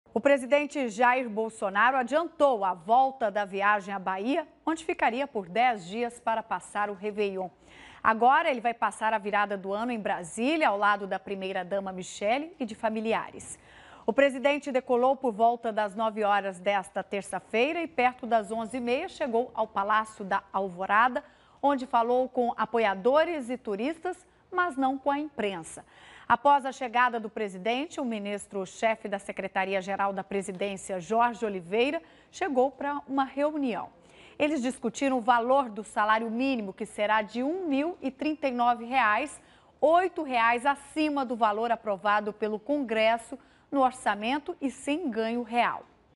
Bolsonaro assina medida e salário mínimo será de R$ 1.039 em 2020
O presidente Jair Bolsonaro assinou nesta terça-feira (31) a medida provisória que estabelece o reajuste do salário mínimo, que entrará em vigor em 1º de janeiro. O valor do mínimo será de R$ 1.039, oito reais acima do valor aprovado pelo Congresso no orçamento, que era R$ 1.031. Em relação ao valor de 2019, o reajuste ficou em 4,1%. Inicialmente, o piso nacional para 2020 seria de R$ 1.040. Depois, ele passou para R$ 1.039 e, em novembro, sofreu nova redução chegando a R$ 1.031. Essas alterações ocorrem em função da expectativa de inflação para o ano, que sofre alterações. De acordo com o Ministério da Economia, o reajuste do mínimo preserva o poder de compra do trabalhador. A pasta explica que a diferença de 8 reais em relação ao orçamento aprovado pelo Congresso se deve pela recente alta do preço da carne, que pressionou a inflação, gerando "uma expectativa de INPC mais alto, o que está refletido no salário mínimo de 2020".